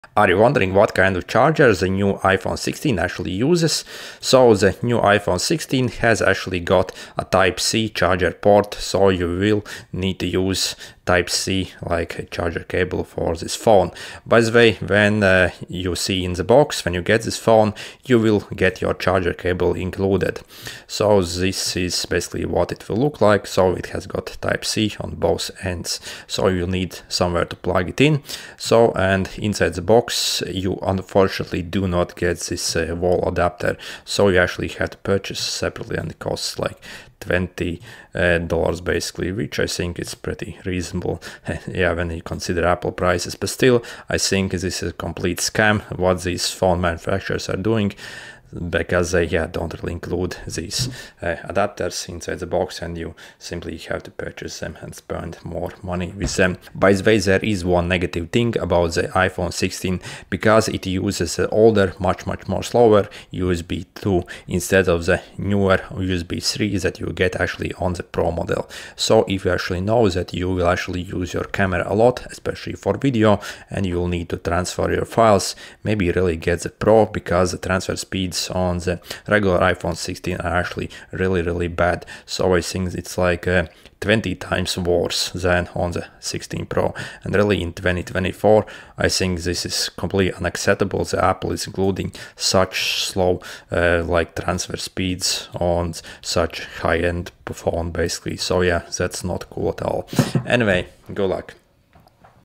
What Charger Port Does Apple Iphone 16 Have? Type-C or Lightning?
Charger Port on Iphone 16 + Transfer Speeds